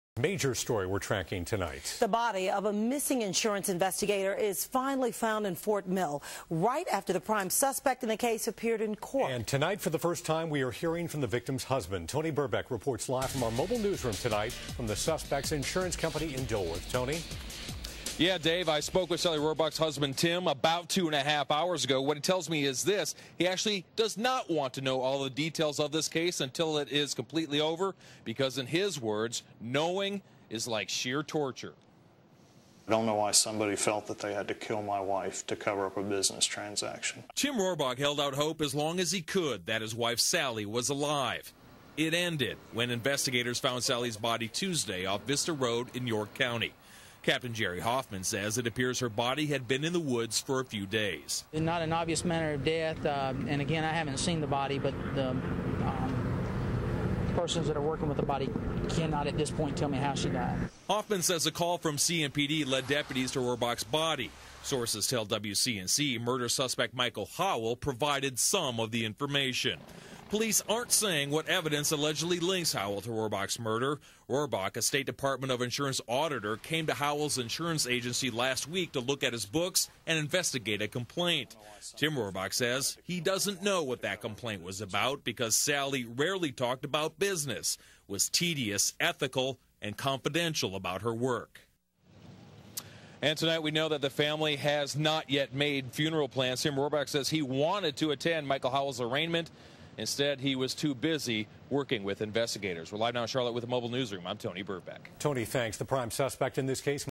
Slain insurance investigator's husband speaks
The husband of a murdered insurance investigator speaks out after authorities locate his wife's body in a wooded area in South Carolina.